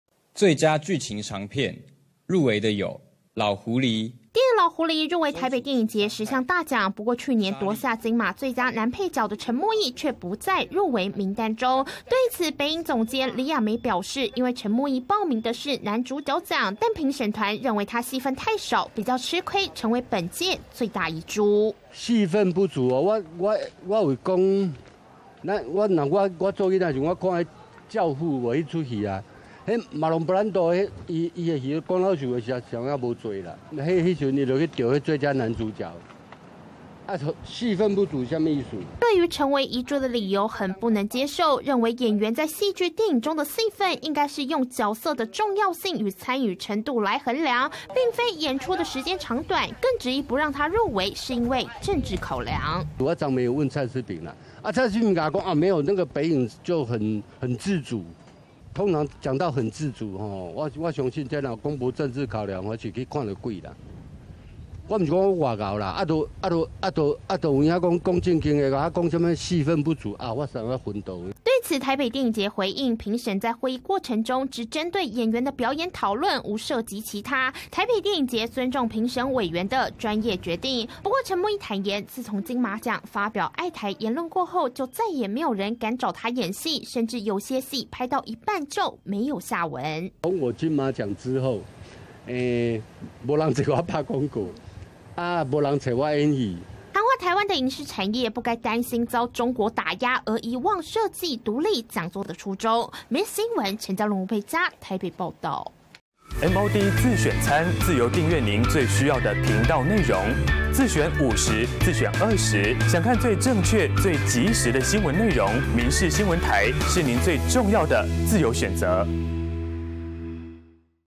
因戲份少成台北電影節最大遺珠 陳慕義轟「政治考量」－民視新聞
【民視即時新聞】演員陳慕義去年以「老狐狸」奪下金馬獎最佳男配角，卻錯失今年台北電影節提名，理由是男主角戲份不足，昨晚陳慕義在臉書發文砲轟，認為「戲份不足」只是藉口，不如直說」、「演戲就演戲搞什麼台獨」，質疑自己的台獨立場才是未入圍的主因。